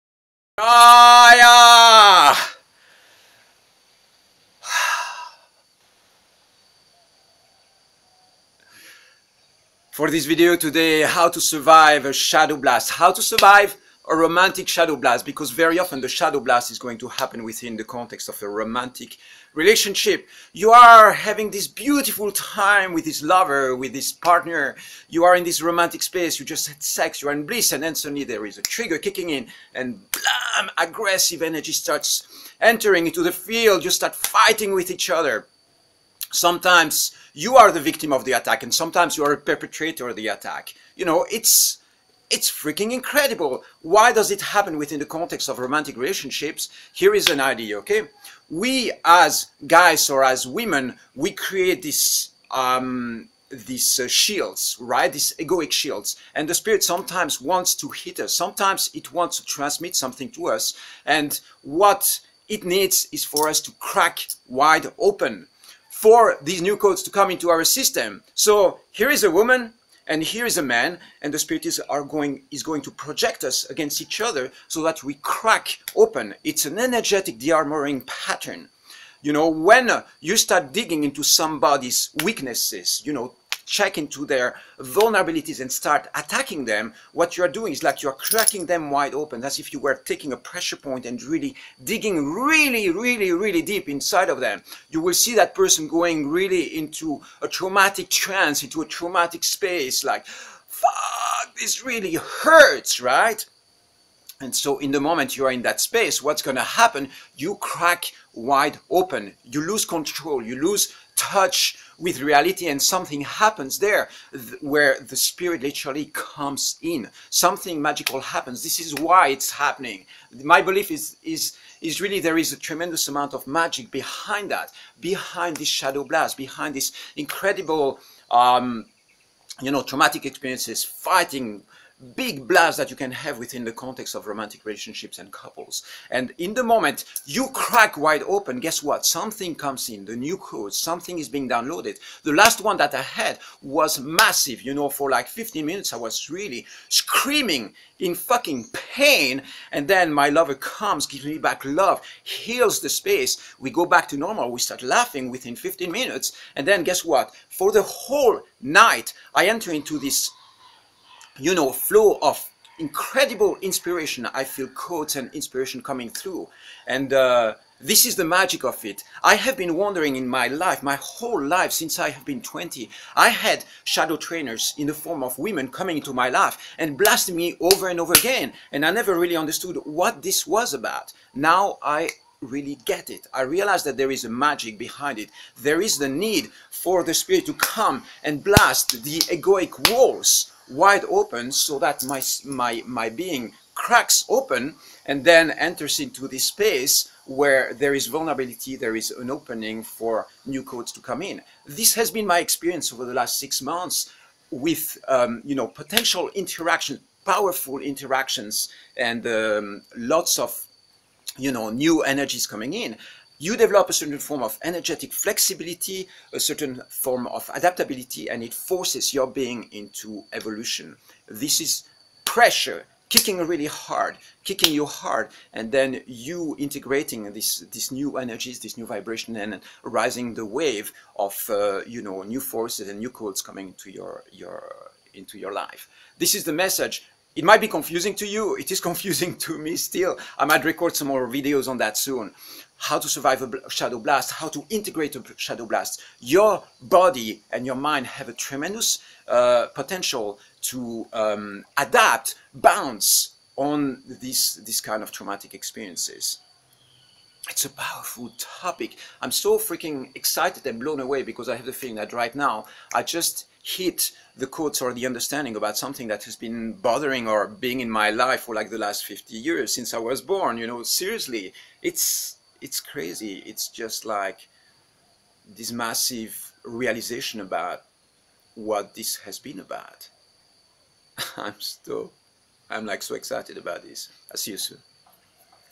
HOW TO SURVIVE A ROMANTIC SHADOW BLAST
Why do couples fight? What happens when you get hit really hard by your lover's extreme rage? How is the spirit using intense dissonance to force you into high speed evolution? Step beyond blame games and tap into the wisdom of these energy dearmoring waves.
Spiritual awakening is not always pleasant or pretty - A fight can crack your egoic walls wide open - Your being fully lets go and receives a refined form of activated grace - That's a shadowgasm - It's a powerful energy breakthrough - New evolutionary codes get transmitted in your system - New powers are unleashed.
This video prepares you for your next shadow outbreak - There is wisdom in every shadow blast - There is an access to light in every shadow! I love you!